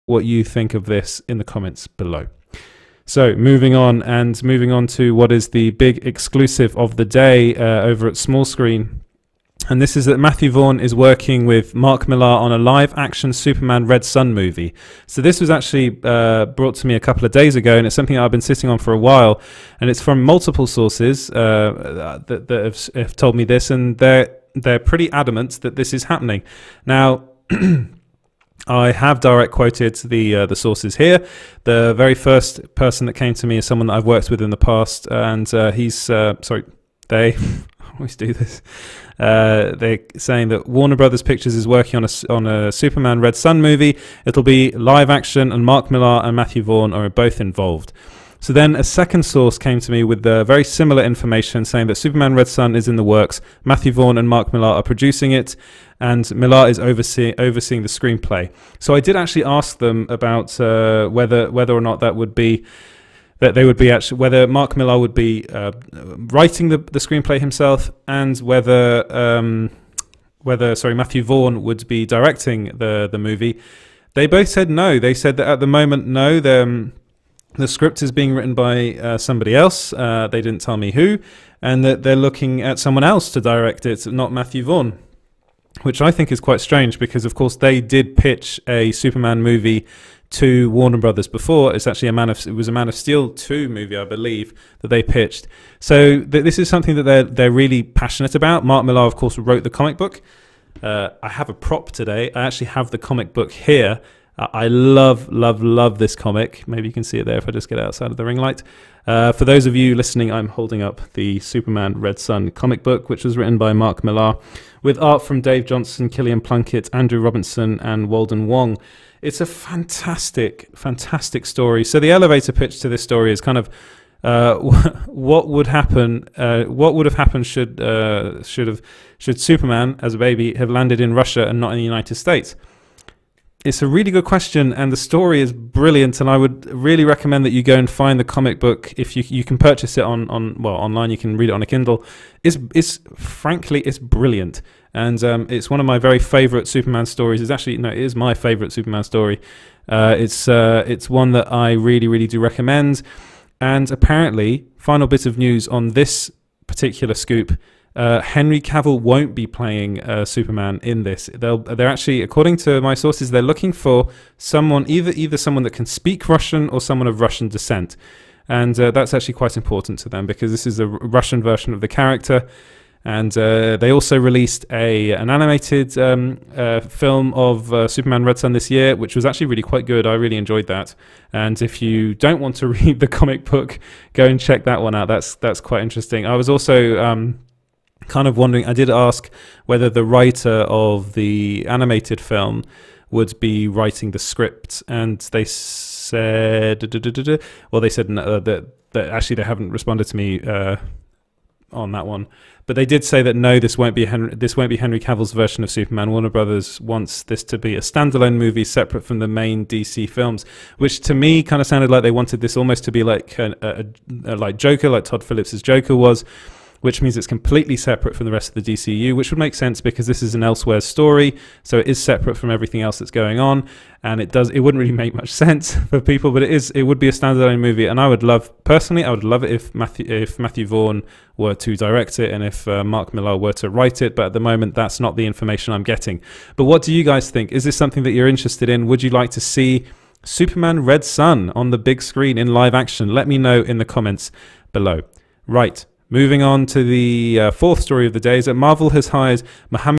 Matthew Vaughn Working With Mark Millar On A Live-Action Superman: Red Son Movie
It’s been revealed to us by multiple sources that a Superman: Red Son movie is in the works with Matthew Vaughn and Mark Millar producing.

This is something that’s come to us from multiple different sources and I didn’t want to write anything before I got some more information first.

Superman: Red Son is a comic book that Mark Millar wrote back in 2003.

It takes the whole mythos of Superman and turns it on its head.

The whole premise of Millar’s Superman story can be boiled down to one central premise: “What if Superman had been raised in the Soviet Union?”

That is such an interesting question to me personally, and I believe it resulted in Millar writing one of the best Superman comic books for all time.

What do you think of this news?

You can read the full EXCLUSIVE story over at Small Screen